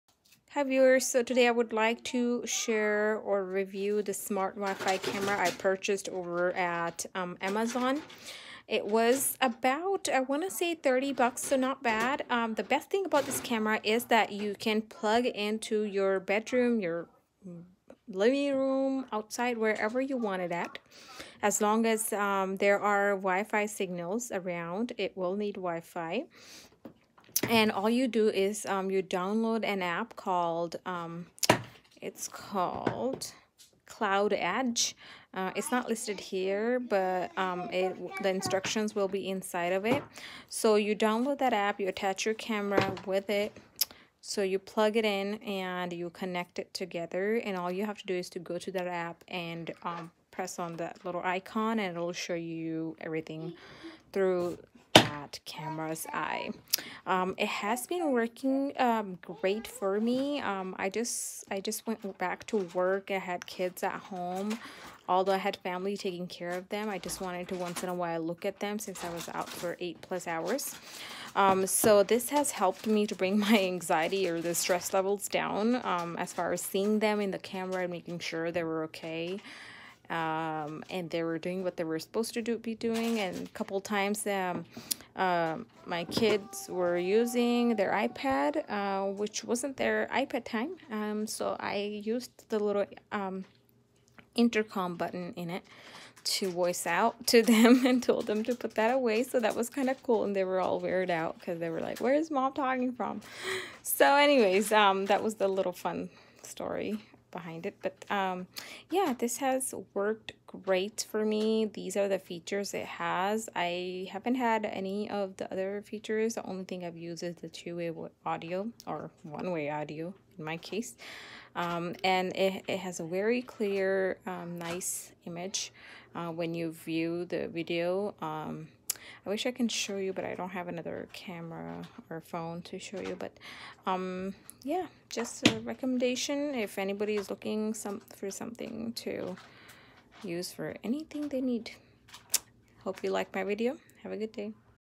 Smart wifi video camera by Meco
I purchased this camera over on Amazon for about 30 bucks. It has worked great for me.